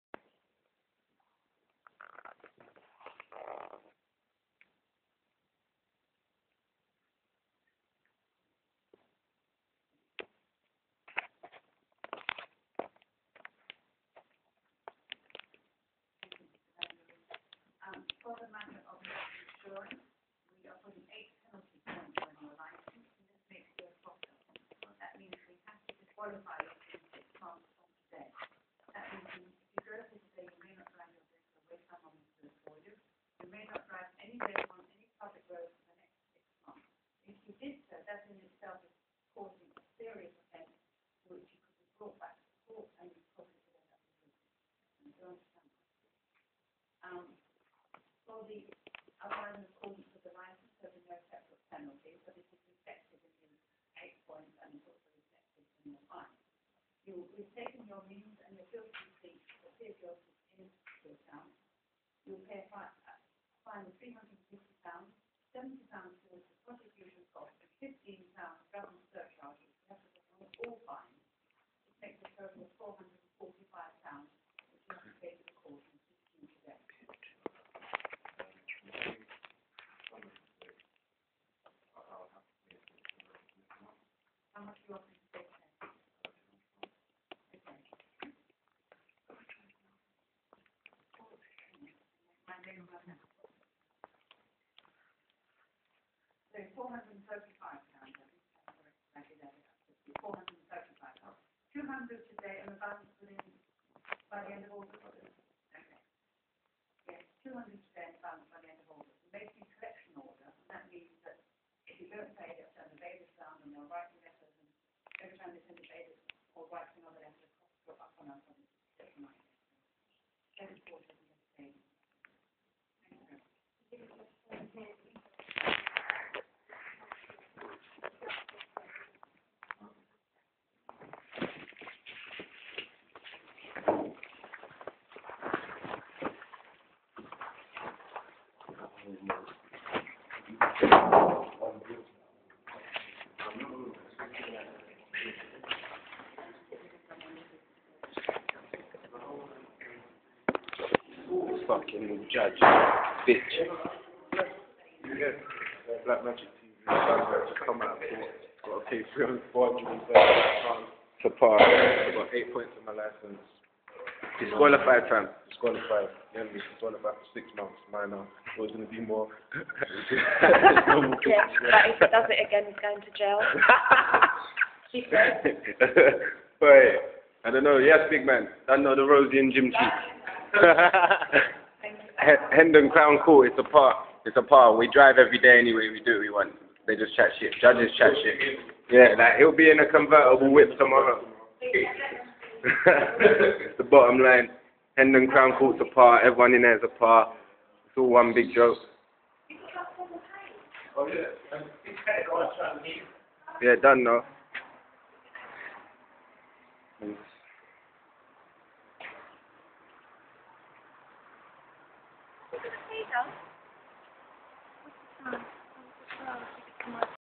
Onyx In Court
Onyx Lost His License LOOL Banned For 6 Months ,Hendon Magistrates Court Wouldnt Let Me Bring My HD In So I Filmed It On The Blackberry. BLACK MAGIK TV!!!